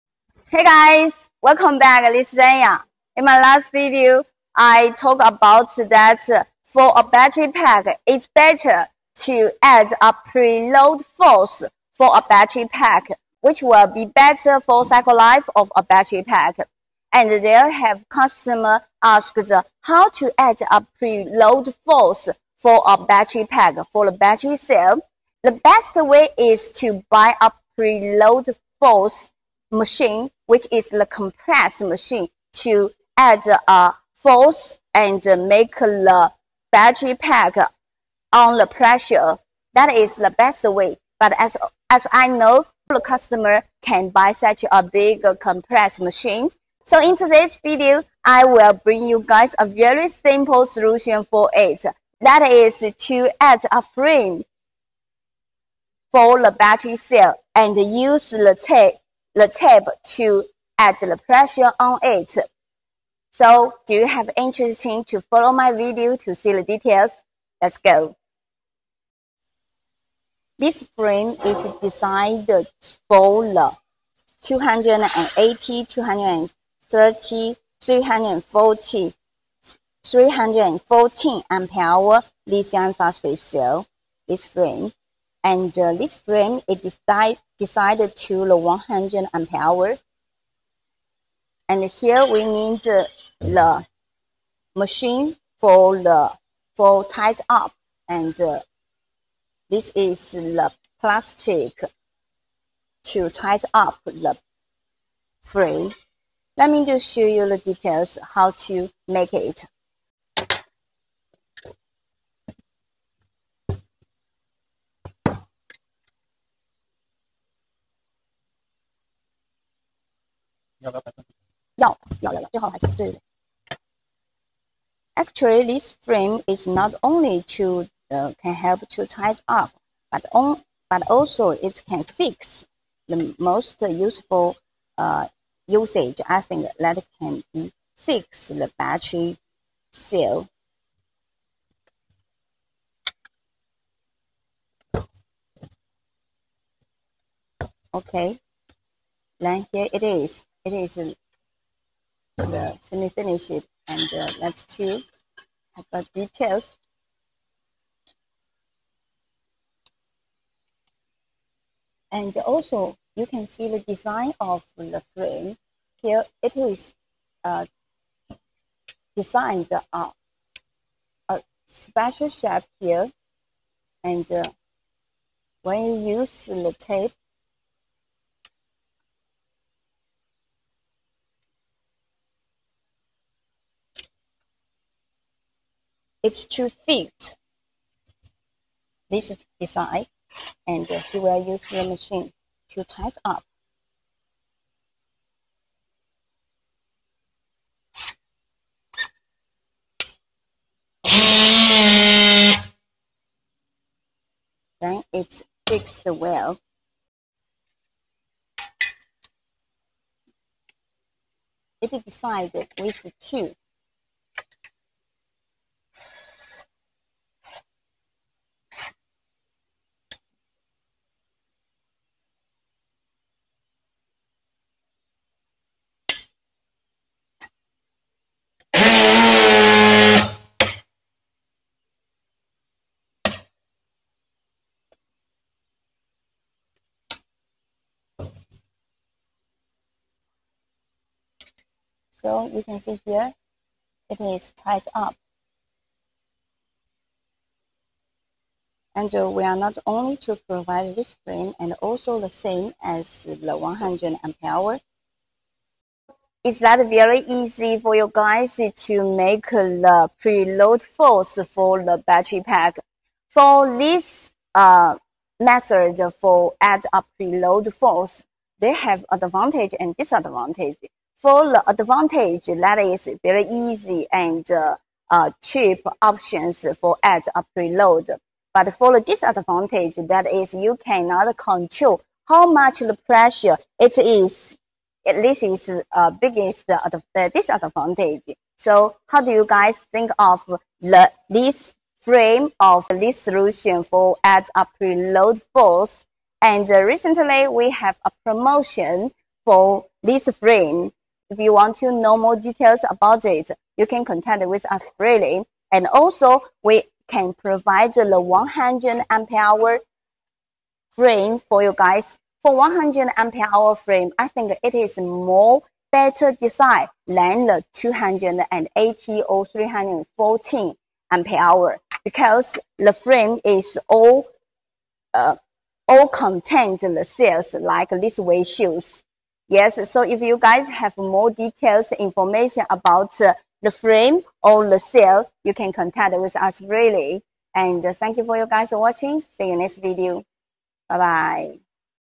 Frame for 314Ah/100Ah to add preload force
This is the easy way for add a preload force for lifepo4 battery, but it is a little hard to control the pressure.
How do you think of this solution?

Frame for 314Ah USD0.99/pc
Order amount exceed $5000 and get free from us !